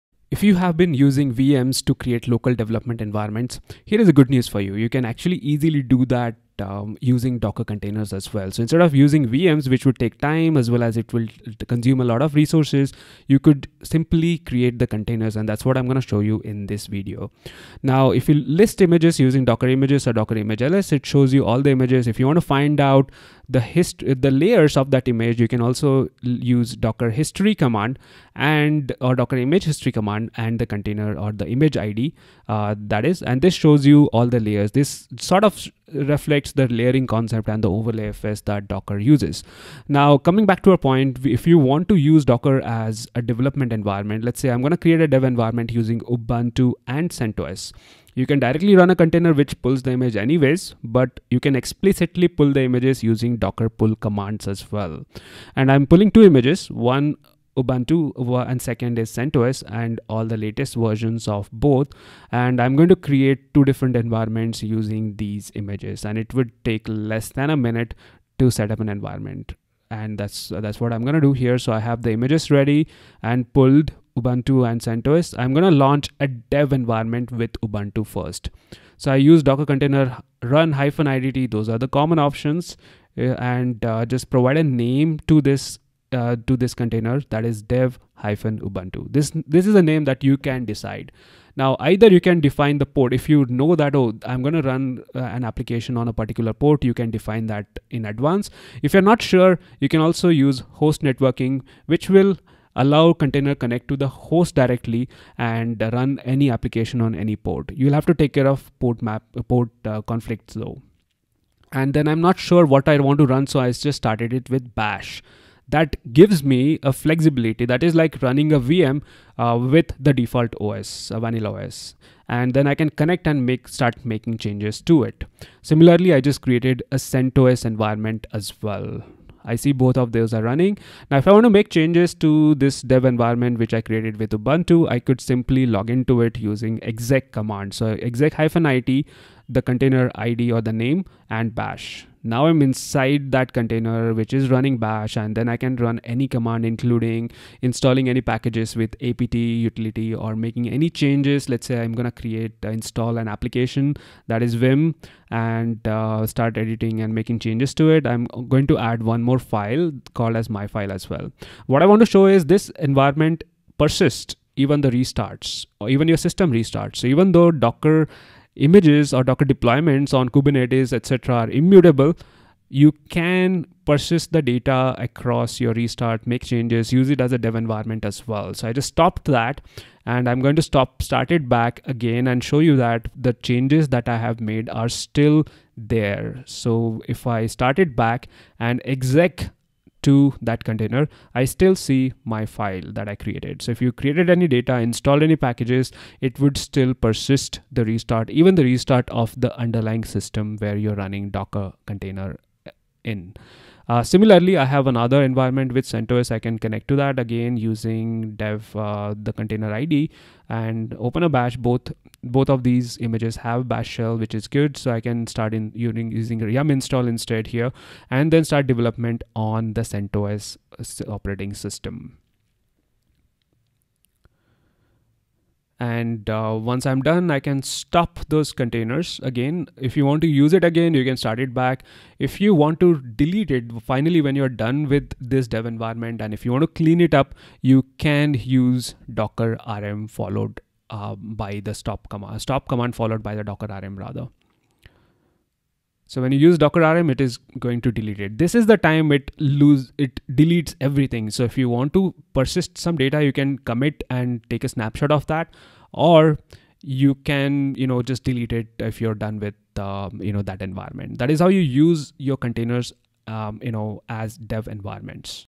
Just enough Docker Part 6:Using docker instead of VMs to create development environments
In this video you will learn how to list images, lunch dev environment cent OS, working inside container, delete dev environment,

Learn devops for FREE with our 24 days Ultimate Devops Bootcamp

Our backpacks give you a targeted learning paths with bundled, structured content.